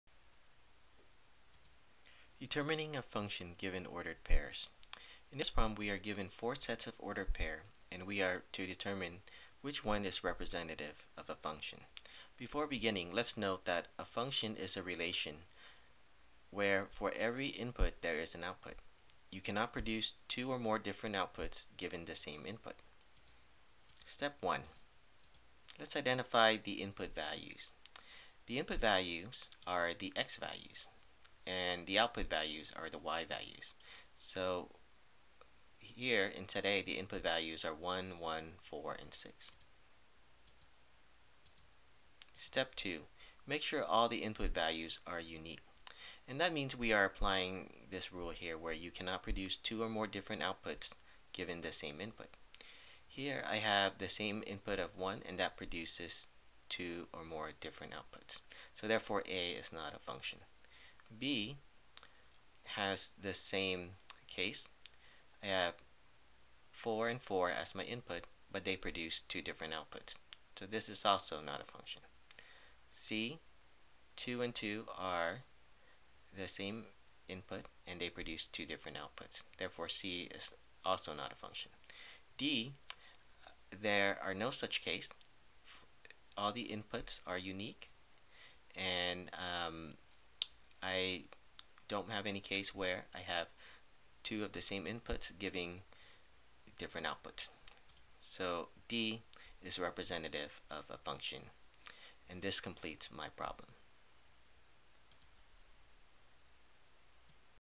How to Determine a function given ordered pairs.
- How to Determine a function given ordered pairs.. For more practice and to create math worksheets, visit Davitily Math Problem Generator